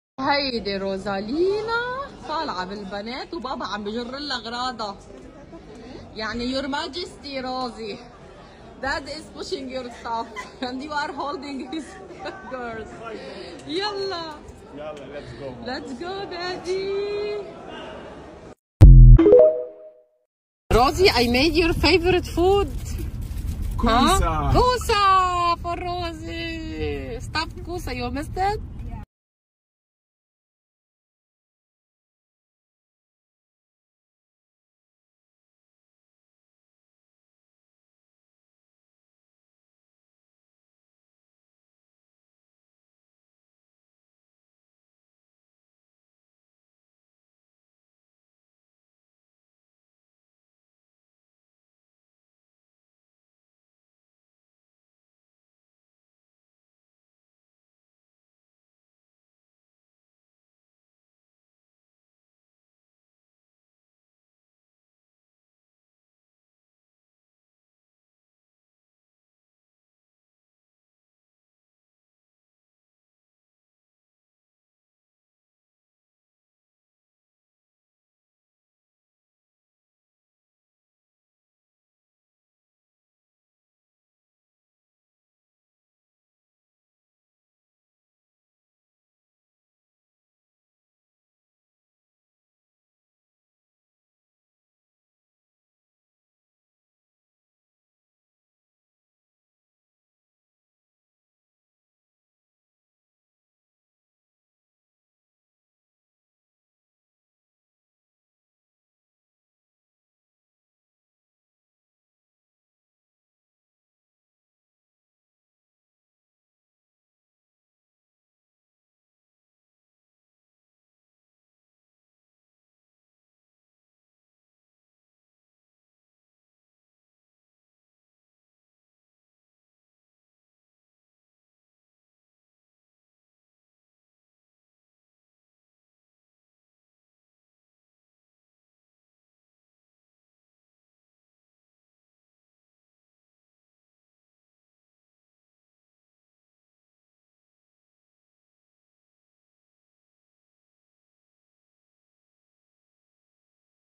WELCOME BACK ROSIE!! Nanny is back to Lebanon Rosie is back to Lebanon
Rosie the Kenyan Nanny has finally returned back to Lebanon